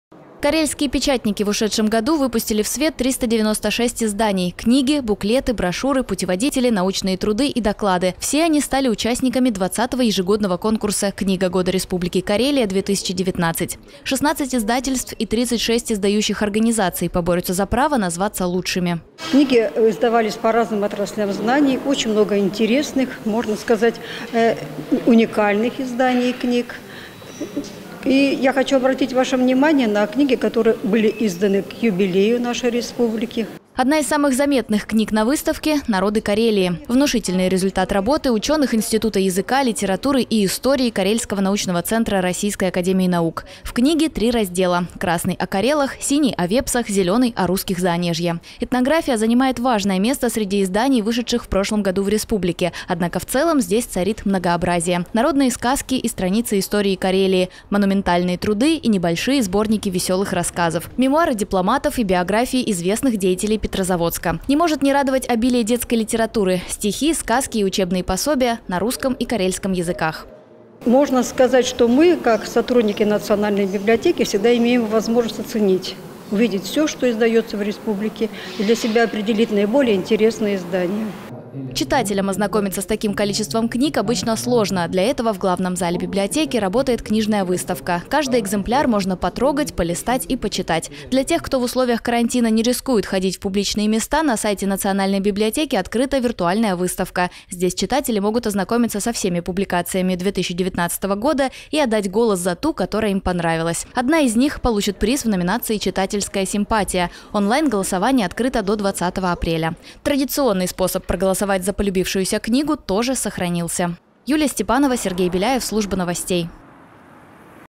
В НАЦБИБЛИОТЕКЕ ПРОШЛО ПЕРВОЕ ЗАСЕДАНИЕ ОРГКОМИТЕТА КОНКУРСА «КНИГА ГОДА РЕСПУБЛИКИ КАРЕЛИЯ - 2019»
Там же открылась одноимённая юбилейная книжная выставка. С некоторыми экспонатами вас познакомит Юлия Степанова.